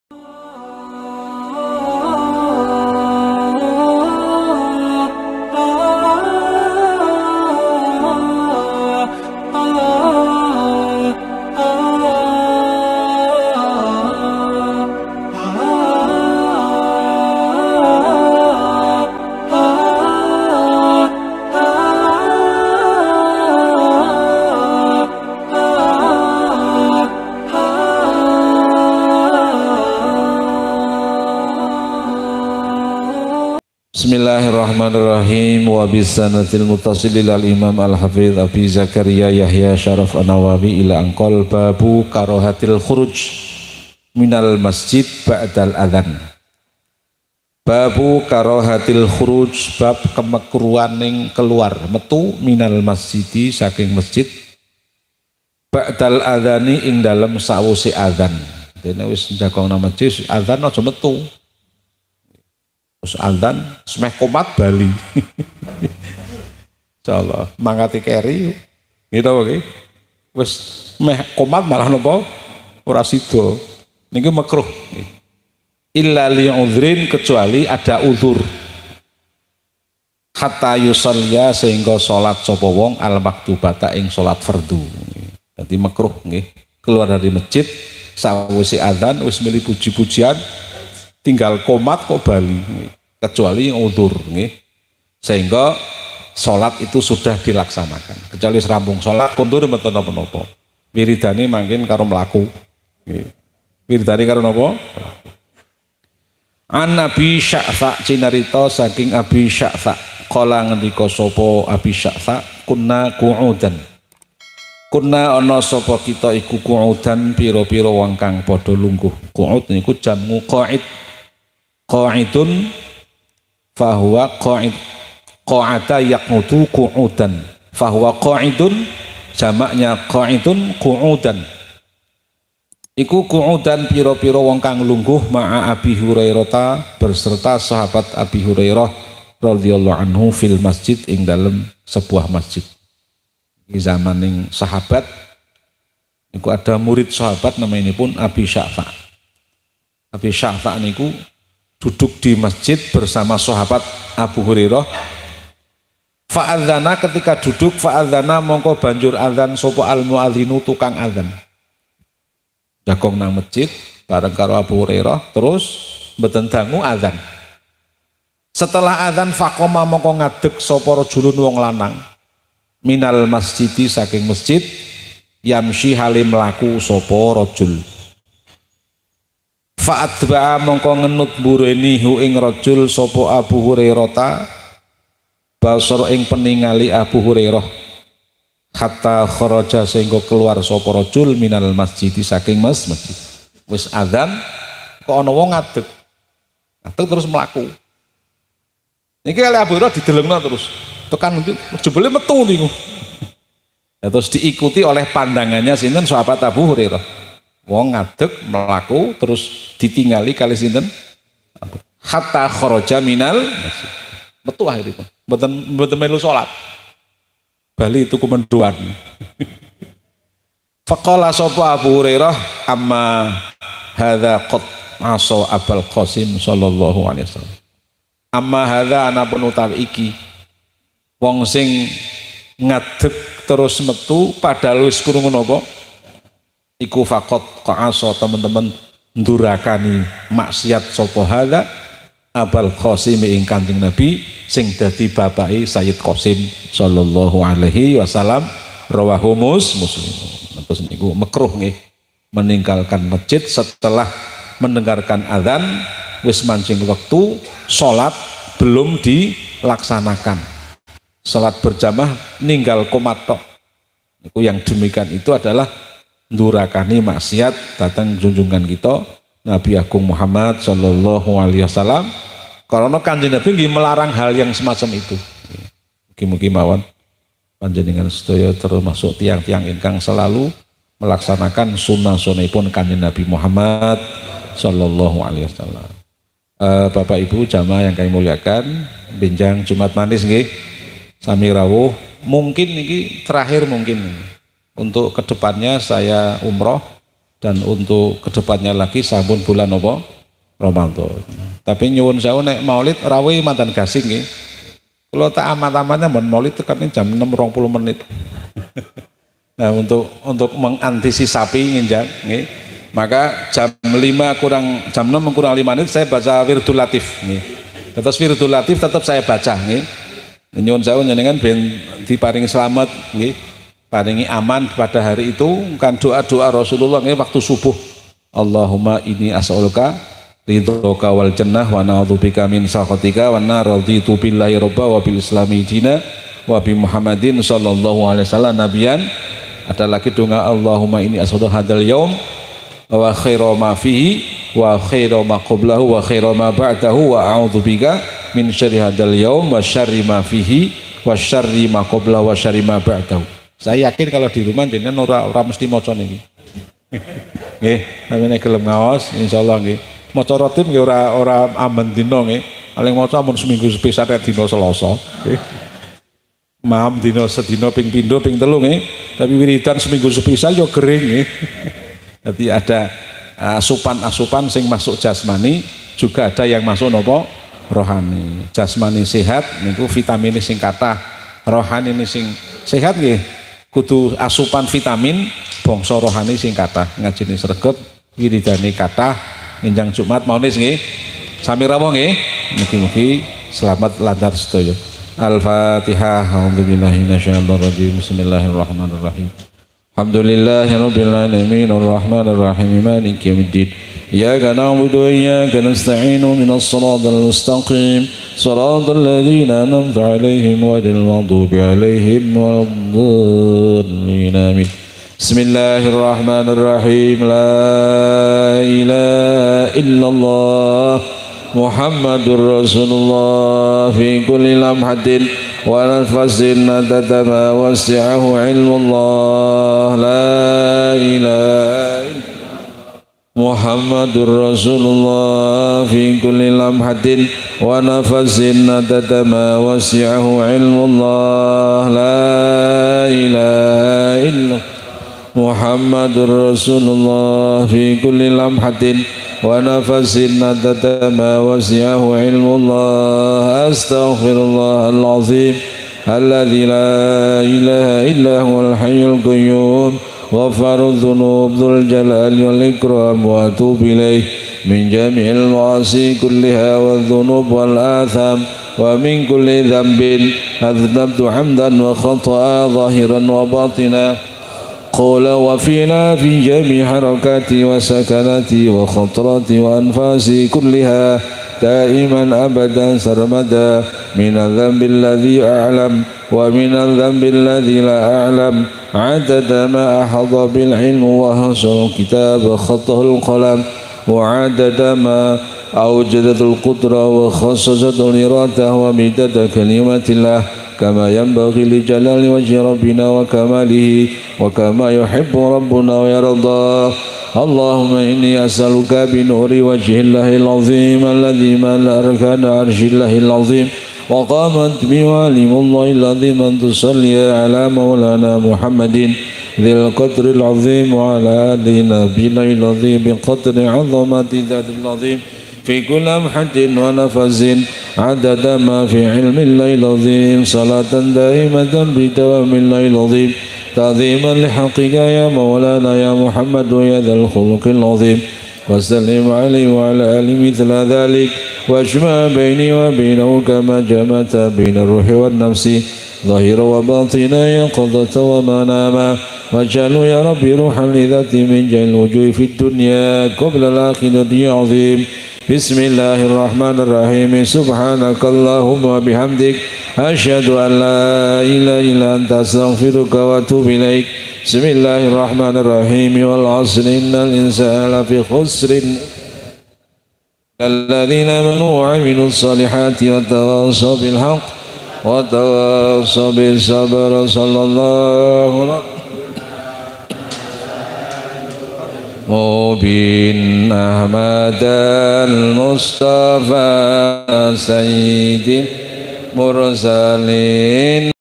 MAKRUH KELUAR DARI MASJID SETELAH ADZAN | mtrs | muslim njawani
Makruh keluar dari masjid ketika adzan sudah dikumandangkan kecuali ada udzur